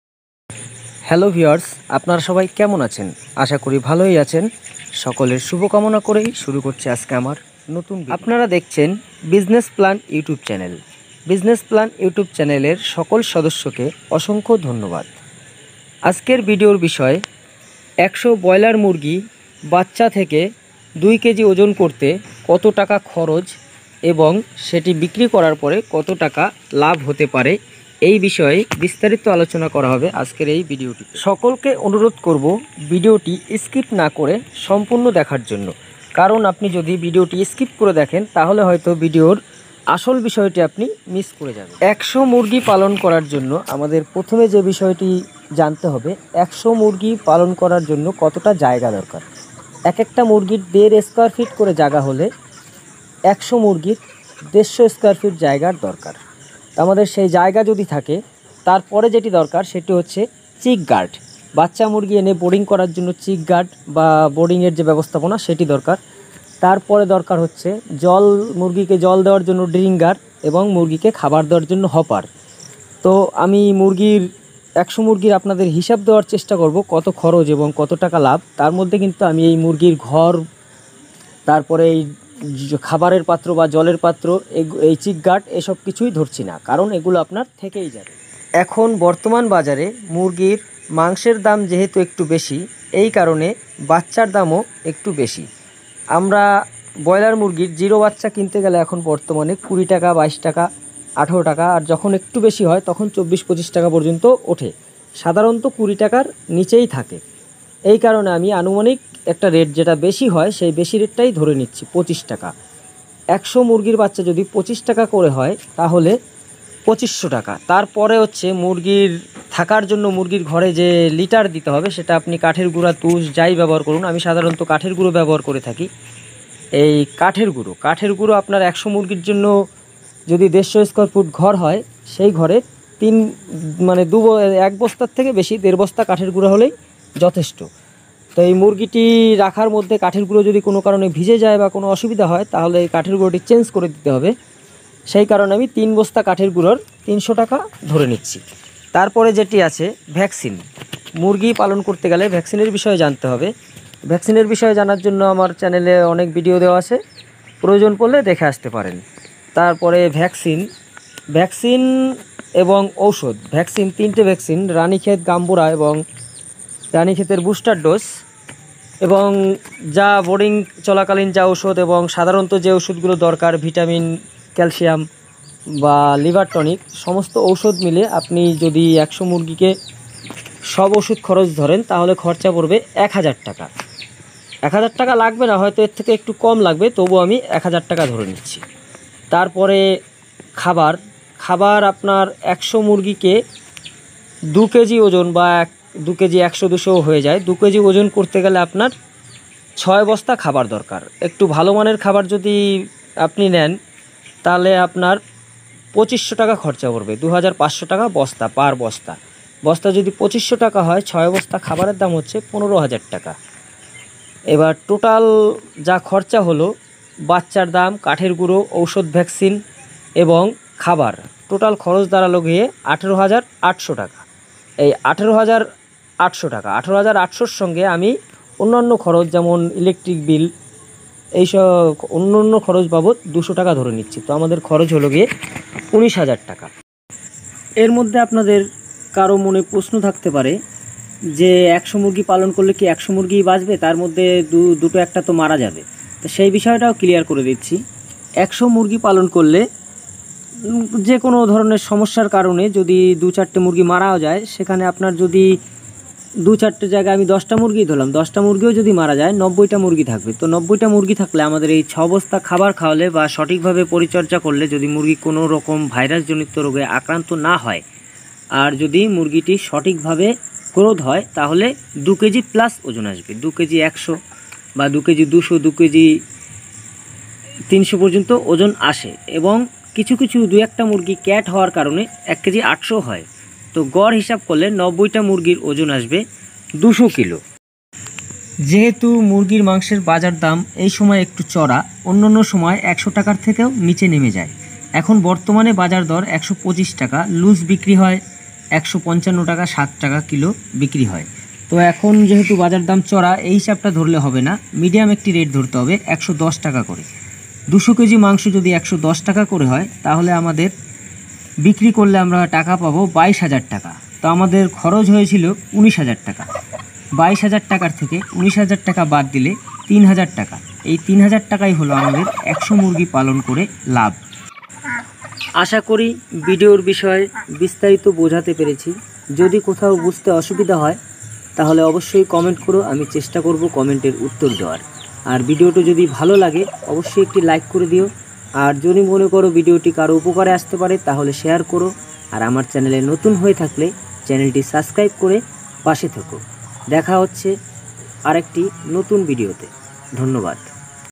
100 বয়লার মুরগি পালন করে কত টাকা খরচ করলে, কত টাকা লাভ হতে পারে!
100 বয়লার  মুরগি পালন করলে কত টাকা খরচ কত টাকা লাভ বিস্তারিত আলোচনা এই ভিডিওটিতে আশা করছেন ভিডিওটি ভালো লাগবে